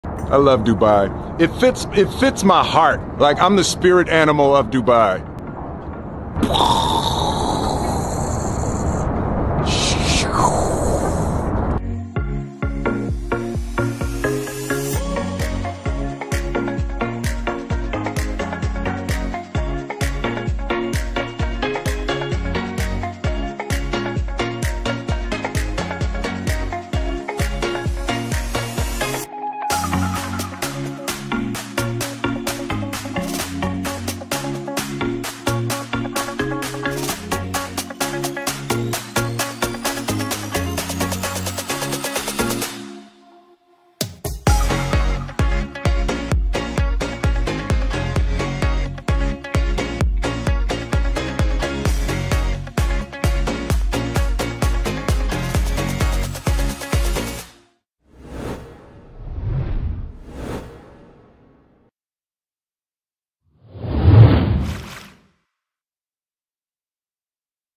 ويل سميث يعشق دبي! 😍😍😍
"أنا أحب دبي!" ويل سميث يزور دبي ويعلن أنها مدينته المفضلة! شاهدوا ماذا فعل هناك ! 😱😍